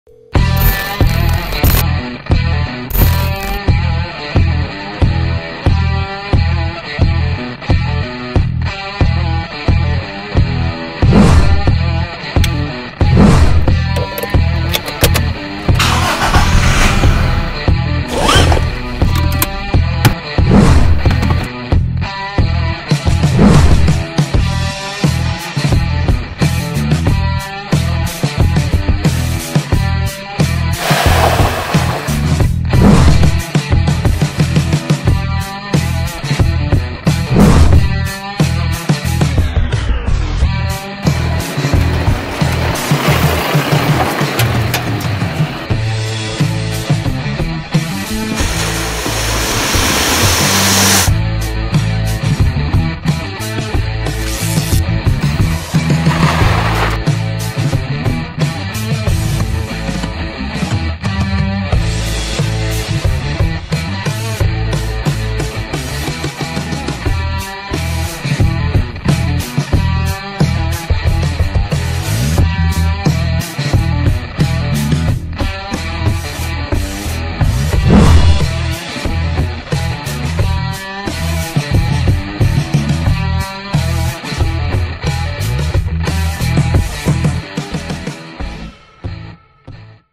2020 TOYOTA HILUX TRD | FILMING AN EPIC CAR COMMERCIAL | CINEMATIC FILM | SONY A6400 | B-ROLL
2020 Toyota Hilux Trd | Hello there! I'm Charles Justin Pasca. This is my first video! I hope you'll like the cinematic film that I did on my Uncle's Toyota Hilux! Camera that I used is Sony A6400.